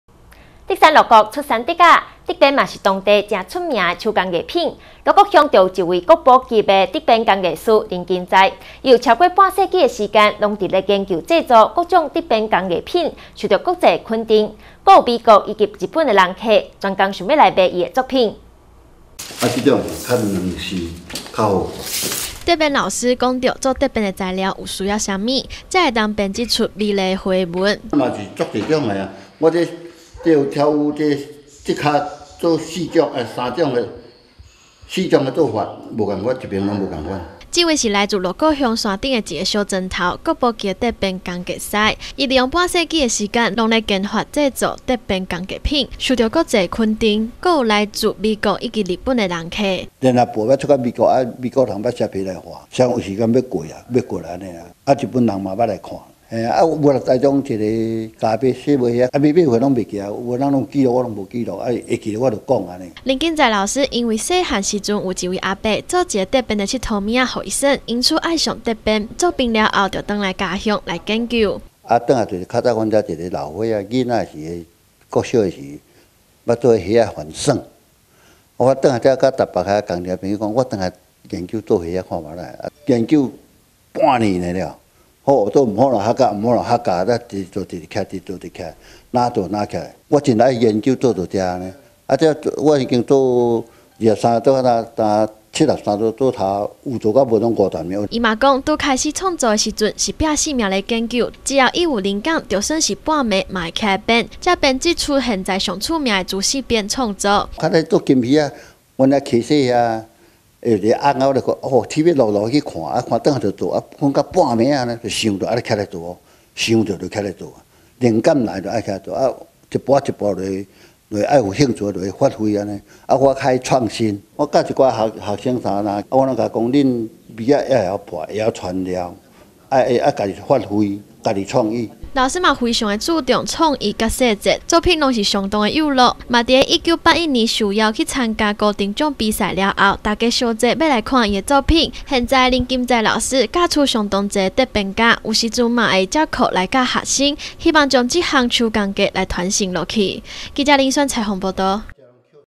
南投新聞 鹿谷國寶級竹編工藝師林根在
竹山鹿谷盛產竹筍，是大家攏知道的事情，但是會如何將竹子變成藝術品，不是件簡單的事情，位置鹿谷鄉山上的一個小村莊，住著一位，國寶級的竹編工藝師，他半世紀的時間，攏在研發製作竹編工藝品，受到國際的肯定，夠有美國以及日本的客人，想要買的他作品，就呼我們來看看，這藏在山中的國寶級工藝。